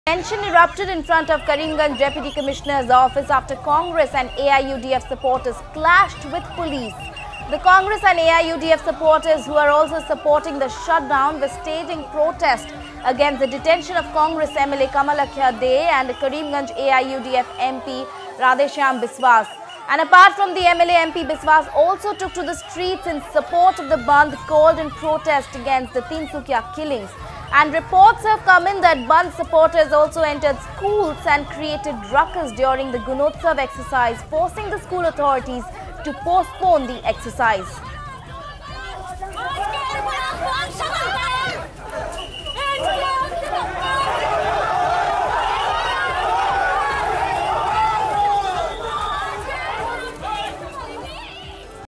Cong, AIUDF bandh supporters clash with police in Karimganj
The Congress and AIUDF supporters who are also supporting the shutdown were staging protest against the detention of Congress MLA Kamalakhya Dey Purkayastha and Karimganj AIUDF MP Radheshyam Biswas.
Tension erupted in front of the Karimganj Deputy commissioner's office after Congress and AIUDF supporters clashed with police.